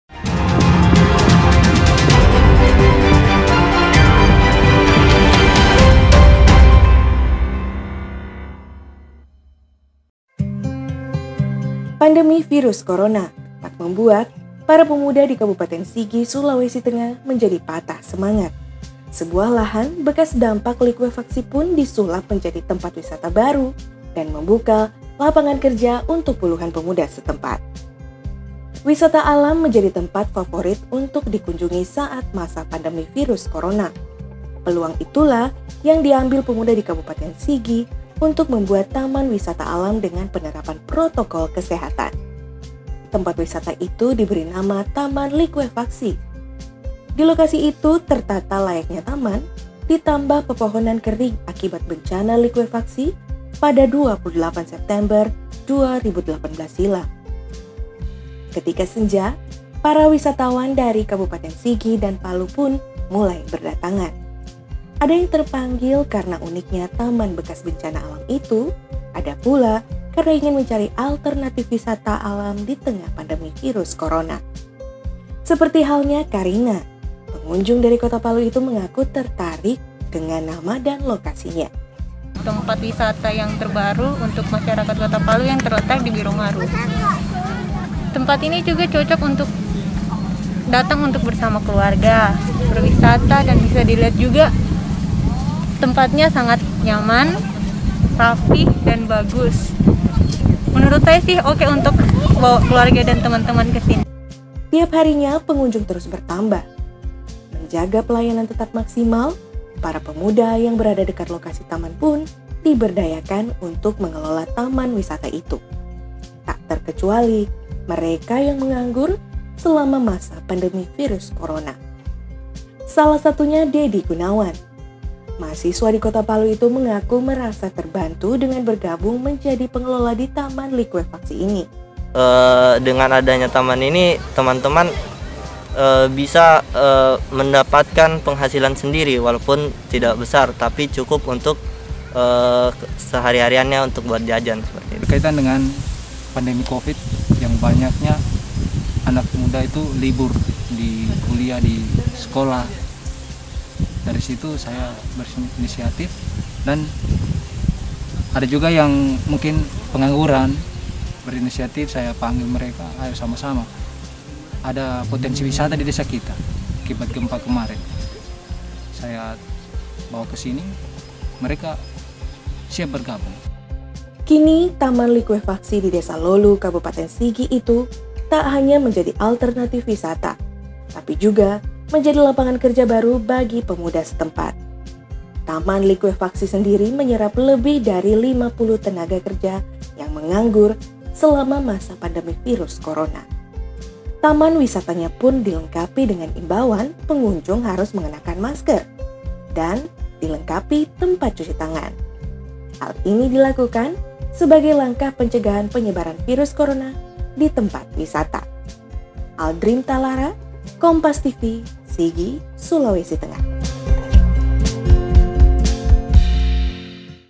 Taman Likuefaksi, Lapangan Kerja Di Tengah Pandemi Covid 19
SIGI, KOMPAS.TV - Pandemi virus korona tak membuat para pemuda di Kabupaten Sigi, Sulawesi Tengah menjadi patah semangat, sebuah lahan bekas dampak likuefaksi pun disulap menjadi tempat wisata baru dan membuka lapangan kerja untuk puluhan pemuda setempat.

Wisata alam menjadi tempat favorit untuk dikunjungi saat masa pandemi virus korona, peluang itulah diambil pemuda di kabupaten sigi untuk membuat taman wisata alam dengan penerapan protokol kesehatan, tempat wisata itu diberi nama taman likuefaksi, di lokasi itu tertata layaknya taman ditambah pepohonan kering akibat bencana likuefaksi pada 28 september 2018 silam.

Ketika senja, para wisatawan dari Kabupaten Sigi dan Palu pun mulai berdatangan, ada yang terpanggil karena uniknya taman bekas bencana alam itu, ada pula karena ingin mencari alternatif wisata alam ditengah pandemi virus korona, seperti halnya Karina, pengunjung dari kota palu ini mengaku tertarik dengan nama dan lokasinya

Tiap harinya pengunjung terus bertambah, menjaga pelayanan tetap maksimal, para pemuda yang berada dekat lokasi taman pun diberdayakan untuk mengelola taman wisata itu, tak terkecuali mereka yang menganggur selama masa pandemi virus korona, salah satunya Dedi Gunawan, mahasiswa di kota palu ini mengaku merasa terbantu dengan bergabung mnjadi pengelola di taman likuefaksi ini.

Kini taman likuefaksi di desa lolu kabupaten sigi itu tak hanya menjadi alternatif wisata, tapi juga menjadi lapangan kerja baru bagi pemuda setempat, taman likuefaksi sendiri menyerap lebih dari lima puluh tenaga kerja yang menganggur selama masa pandemi virus korona.

Taman wisatanya pun dilengkapi dengan imbauan pengungjung harus mengenakan masker, dan dilengkapi tempat cuci tangan, hal ini dilakukan sebagai langkah pencegahan penyebaran virus korona di tempat wisata.